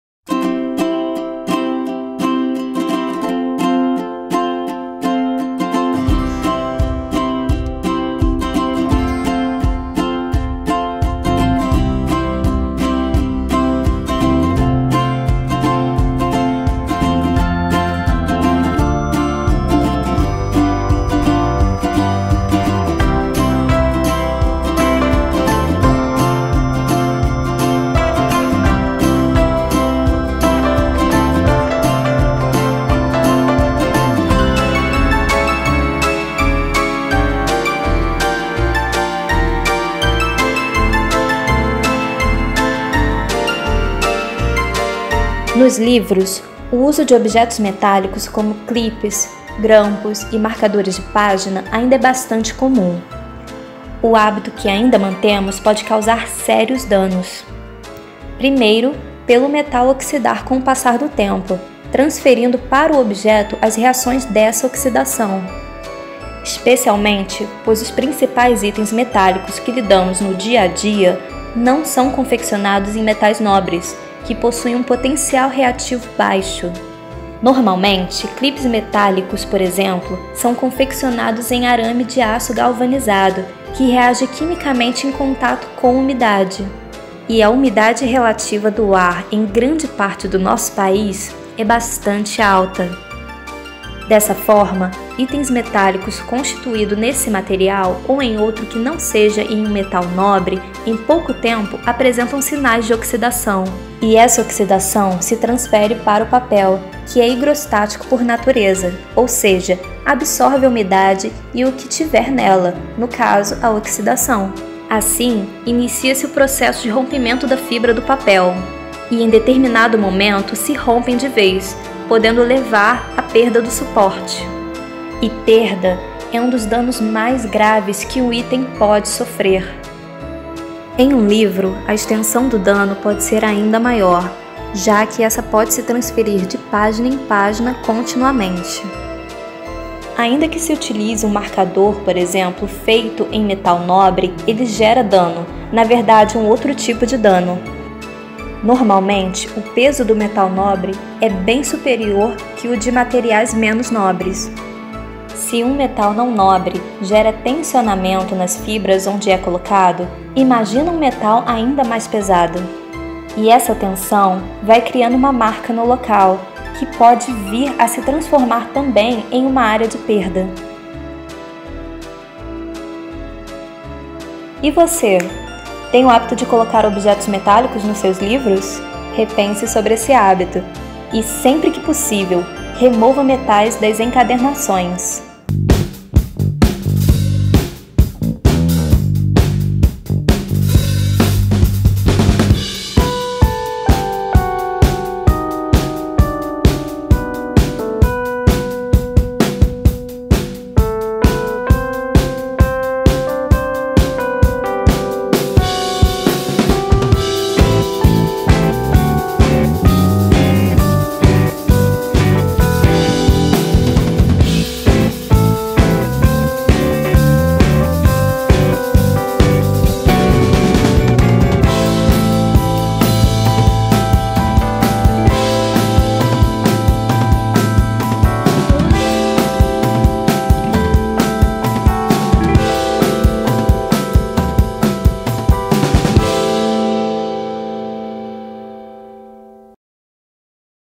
Remoção de Metais: Livros | Semana 3 | 6 Semanas de Conservação Preventiva
“6 Semanas de Conservação Preventiva” traz dicas de como conservar
documentos e livros.

O projeto é composto por 12 vídeos, produzidos pelo
Laboratório de Conservação e Restauração do Museu
Imperial, e explora os acervos do Arquivo Histórico e
Biblioteca da instituição.

➡Acompanhe o Museu Imperial através da internet!